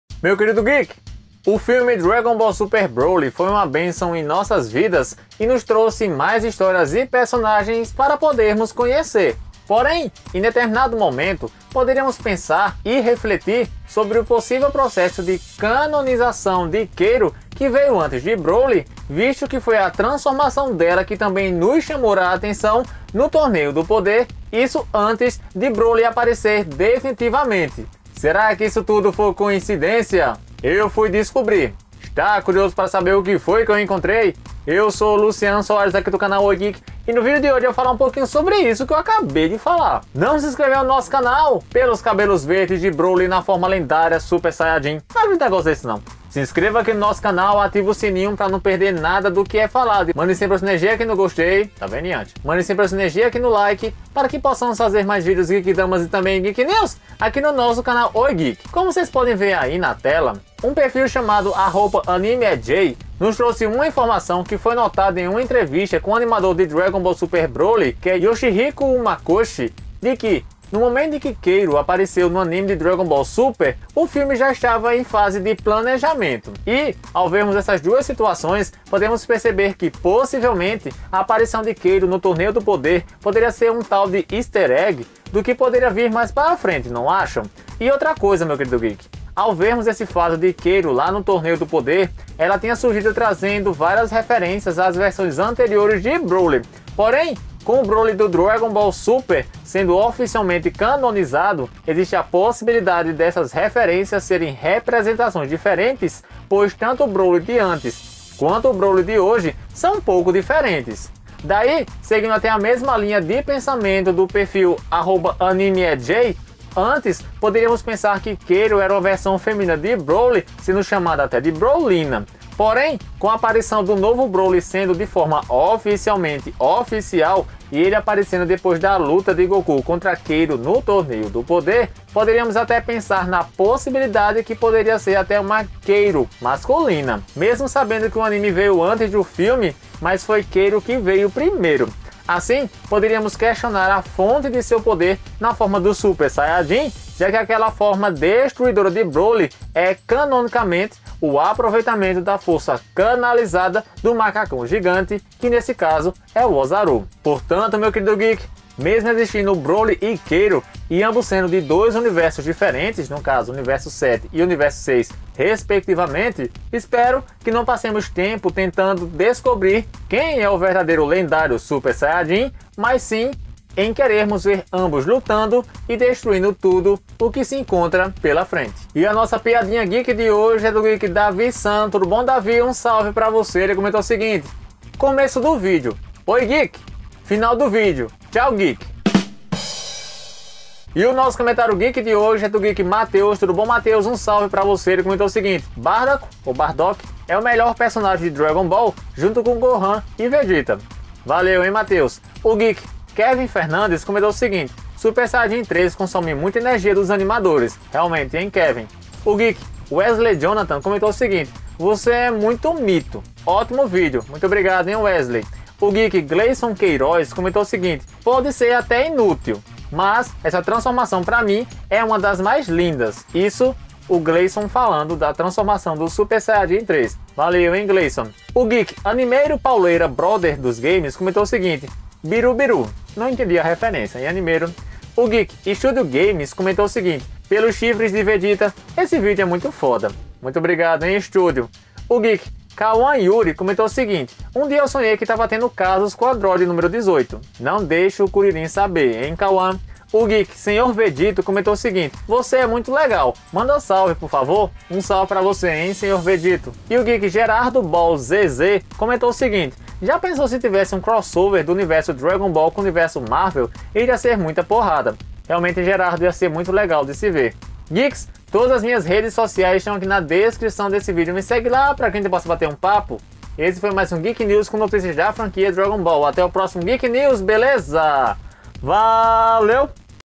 KALE MASCULINA? BROLY FEMININO? NOVO LENDÁRIO SUPER SAIYAJIN? BROLINA EM DRAGON BALL SUPER?
* Se inscrevam nos meus dois Canais *